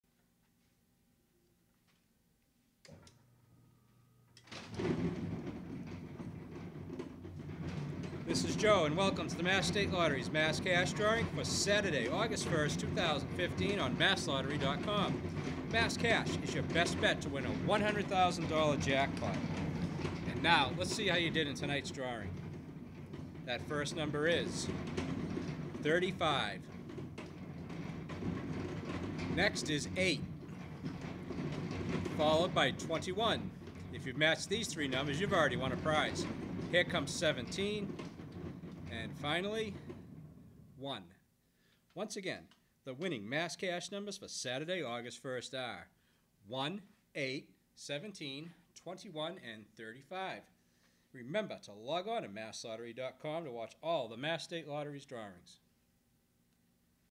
Mass Cash Game Drawing: Saturday, August 1, 2015
The official winning numbers in the Mass. State Lottery's Mass Cash Game drawing for Saturday, August 1, 2015 are:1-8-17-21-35.